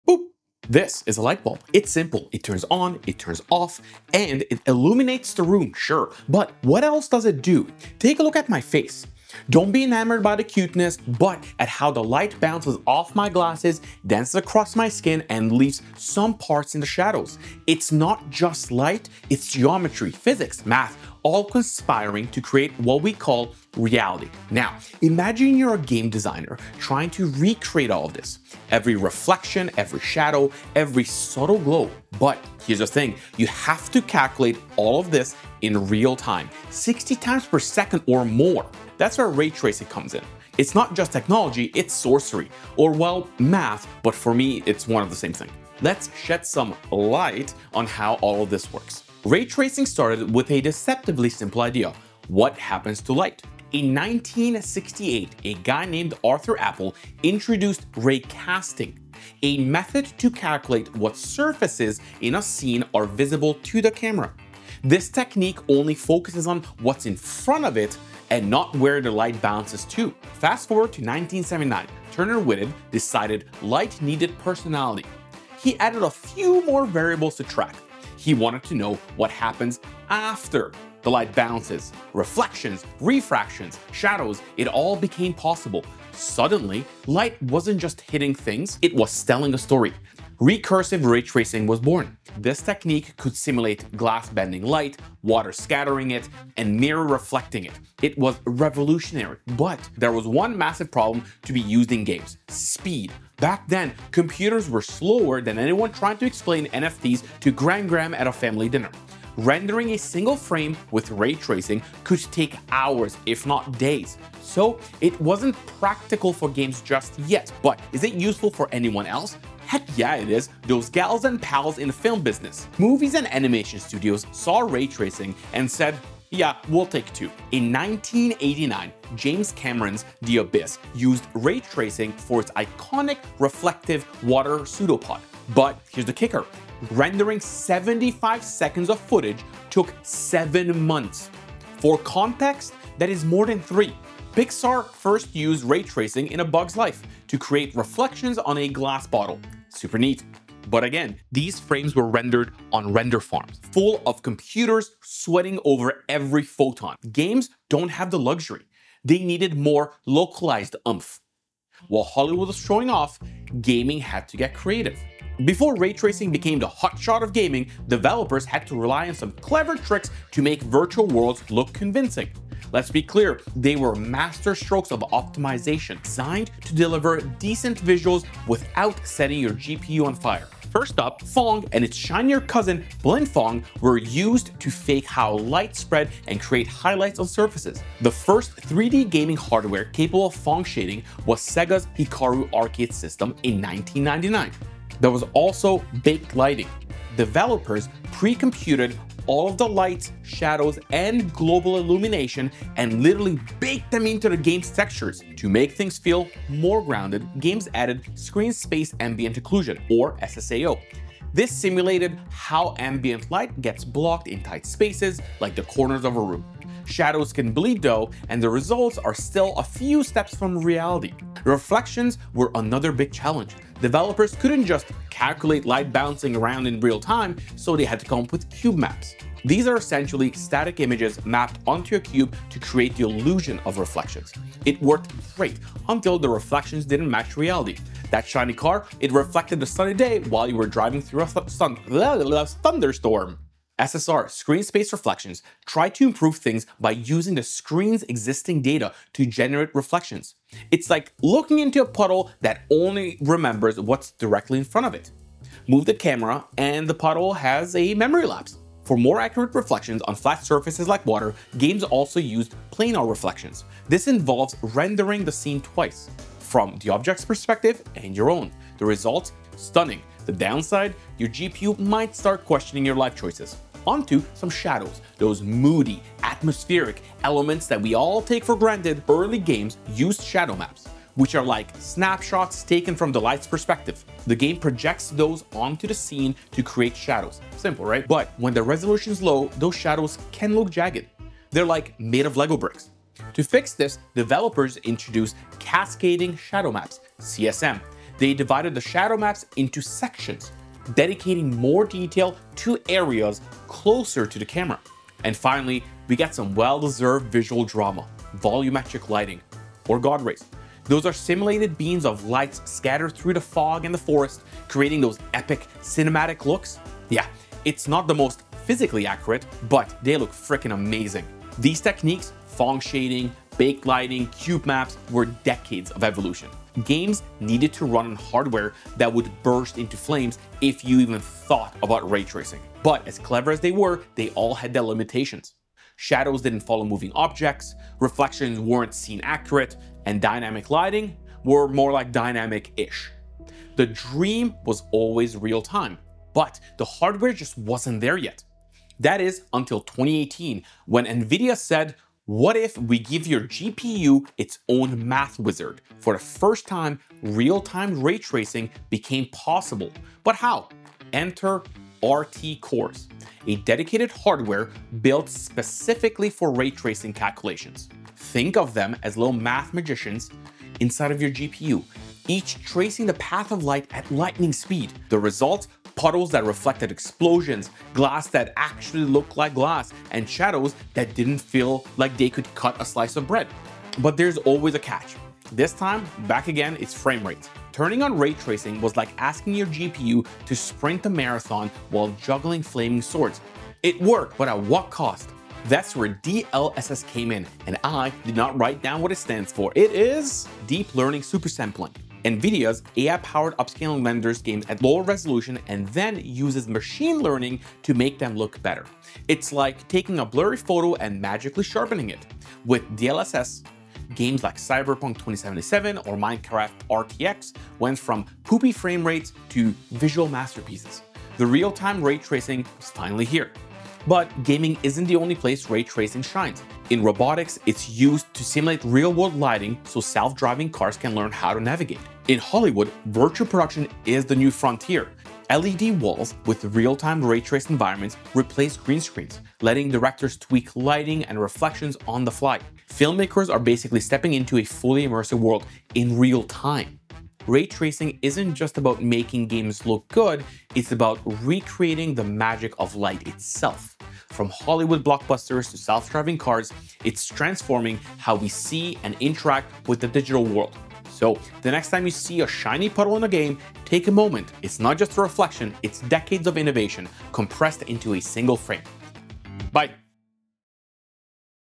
80% accurate story of Ray Tracing
Chapter
0:00 - 0:40 Intro
0:41 - 2:00 History of Ray tracing
2:01 - 2:44 Ray Tracing in Movies
2:45 - 5:49 Techniques before ray tracing
5:50 - 6:35 Ray tracing now
6:36 - 7:07 DLSS
7:08 - 7:51 Conclusion

"Ever wondered how games and movies make light feel real? Ray tracing is the answer. It’s the groundbreaking technology behind the jaw-dropping visuals in modern games like Cyberpunk 2077 and Minecraft RTX, and it’s shaping industries far beyond gaming.

Side note: new Indiana Jones and the Great Circle looks SICKI

In this video, we explore the magic of light and how ray tracing recreates the way light bounces, reflects, and interacts with the world. From its origins in 1968 with Arthur Appel’s ray casting, to its Hollywood debut in films like The Abyss & The Bugs Life to its real-time revolution with NVIDIA’s RTX GPUs this is the story of light, pixels, and the tech that bridges them.

We’ll also break down the ingenious techniques developers used before ray tracing (baked lighting, cubemaps, and those infamous screen-space reflections) And it’s not just games, ray tracing is helping self-driving cars, virtual production in Hollywood, and even astrophysics!

If you’ve ever been mesmerized by a shiny puddle, awed by cinematic shadows, or just curious about what makes your GPU sound like it’s trying to launch into orbit, this video is for you.